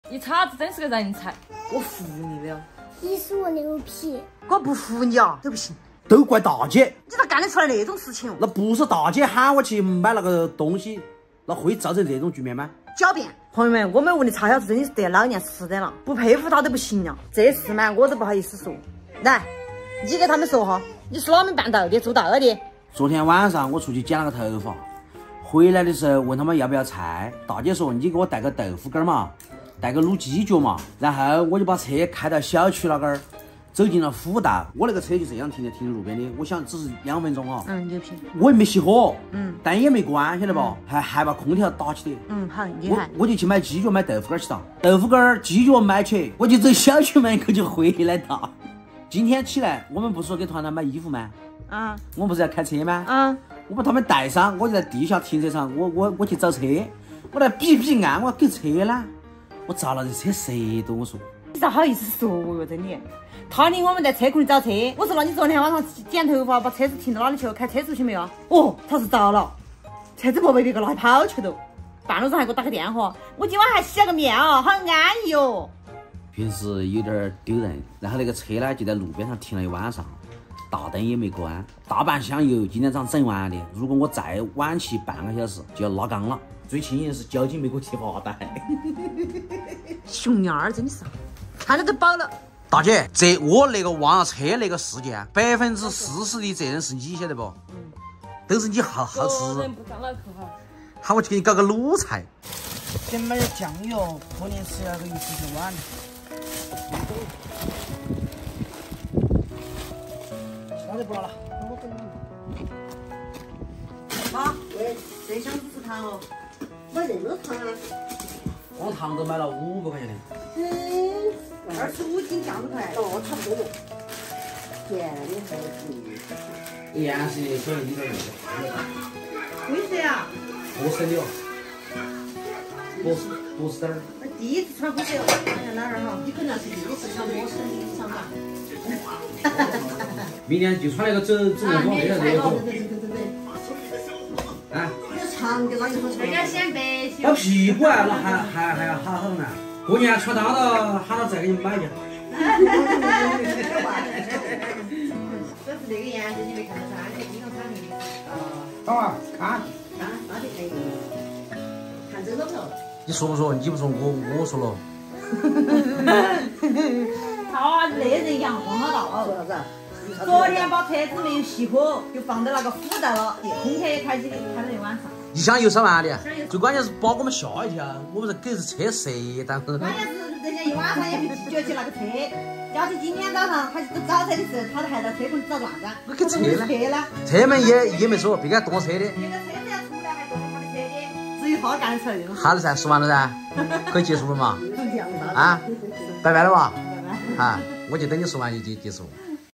老公回家把车忘在路边！给公公买新衣服，将功补过下次再也不能忘记了！【杨翠花V】
哈喽
这里是杨翠花，杨翠花的杨，杨翠花的翠花！🌸
直爽的四川辣妹子！爱哭、爱闹、爱笑、爱交朋友😀

家有“两厨”，超小厨和我的小小厨。